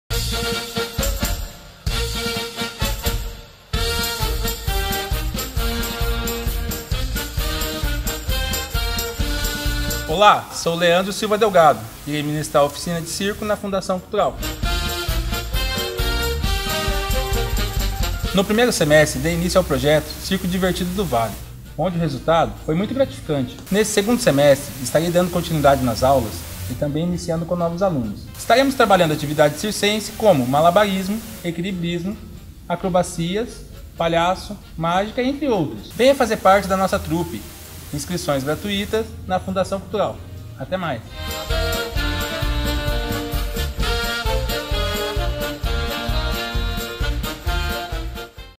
Circo Divertido no Vale
Projetos Culturais | 2º SEM/2015

Leandro Silva Delgado irá dar continuidade ao projeto Circo Divertido no Vale e, também, iniciar as atividades circenses com novos alunos.